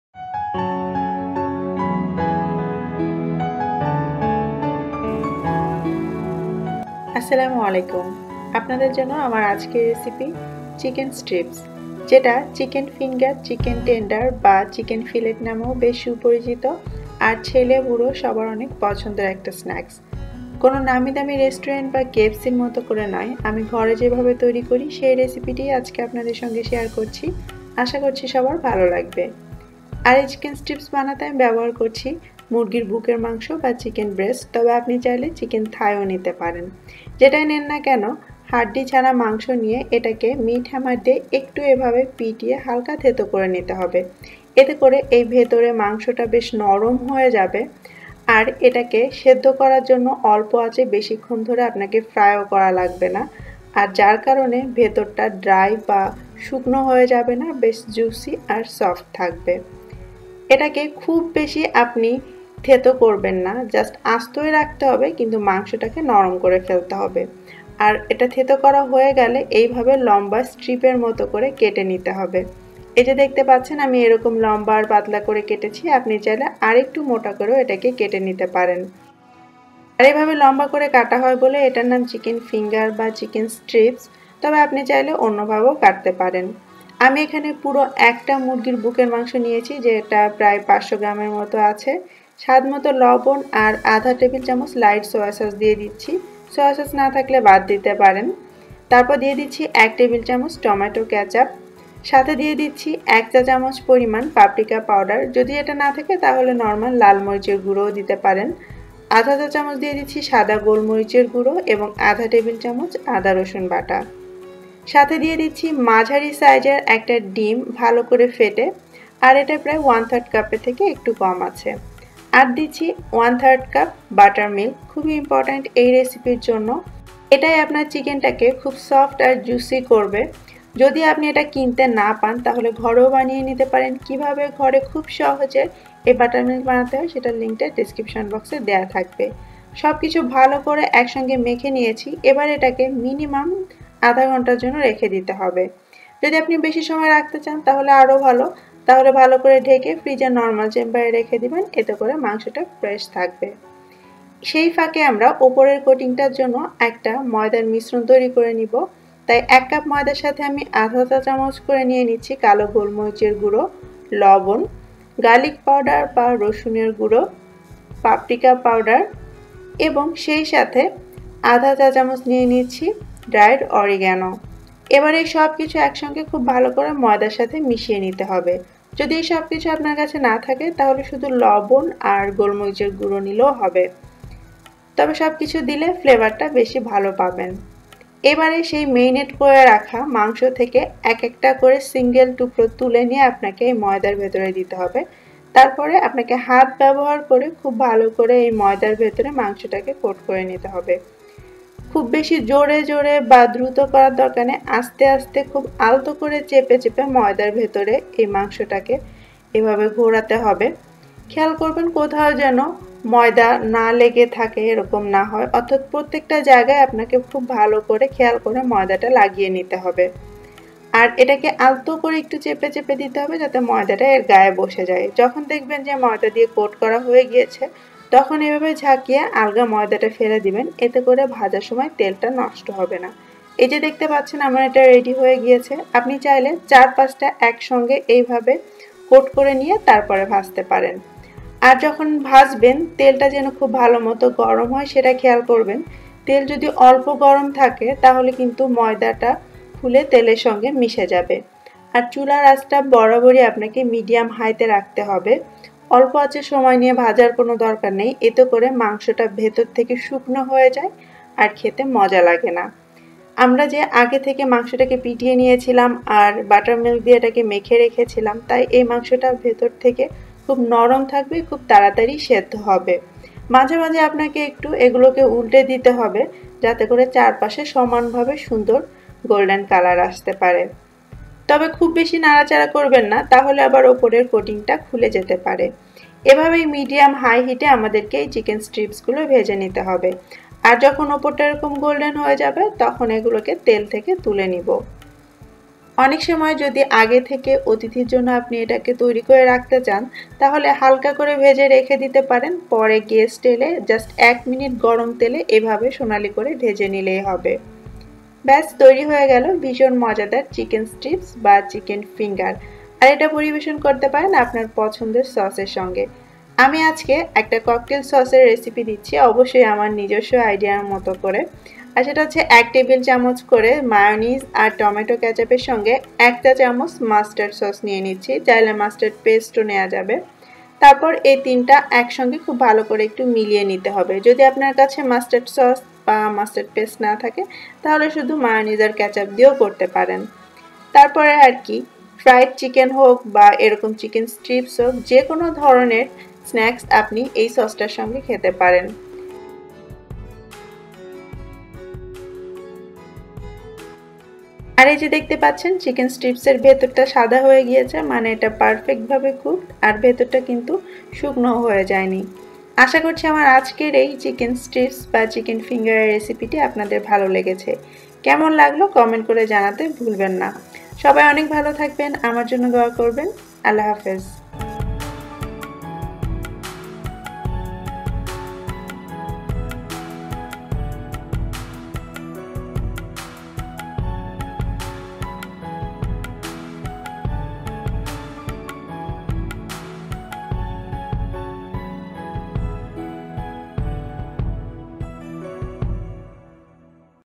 সুস্বাদু চিকেন ফিঙ্গার/স্ট্রিপ্স || How to make Chicken Strips/ Finger/ Tenders Recipe Bangla, KFC
সুস্বাদু চিকেন ফিঙ্গার/স্ট্রিপ্স || How to make Chicken Strips/ Finger/ Tenders Recipe Bangla, KFC chicken fry

চিকেন স্ট্রিপ্স/ ফিঙ্গার /টেন্ডার বা চিকেন ফিলেট যে নামেই ডাকেন না কেন খেতে কিন্তু সেইইইই মজা।  বাচ্চা-বুড়ো সবার অনেক পছন্দের এই স্নাক্সটির রেসিপি নিয়ে আমার আজকের এপিসোড।
বাসায় নিজে যেভাবে করি সেটাই শেয়ার করছি , আশা করছি আপনাদের ভালো লাগবে।

উপকরণ :

চিকেন ব্রেস্ট - ৫০০ গ্রাম
লবন - ১/২ চা চামচ
সয়া সস - ১/২ টেবিল চামচ
টমেটো ক্যাচাপ - ১ টেবিল চামচ
পাপরিকা / মরিচ গুঁড়ো - ১ চা চামচ
গোলমরিচ গুঁড়ো - ১/২ চা চামচ
আদা-রসুন বাটা - ১/২ টেবিল চামচ
ফেটানো ডিম্ - ১ টি
বাটার মিল্ক - ১/৩ কাপ

বাটারমিল্কের রেসিপি 👉 👉 ১ কাপ ফুলক্রিম দুধের সাথে ঠিক ১ চা চামচ ভিনেগার বা লেবুর রস মিলিয়ে ৫ মিনিট অপেক্ষা করুন।  ৫ মিনিট পর আপনার বাটার মিল্ক তৈরী।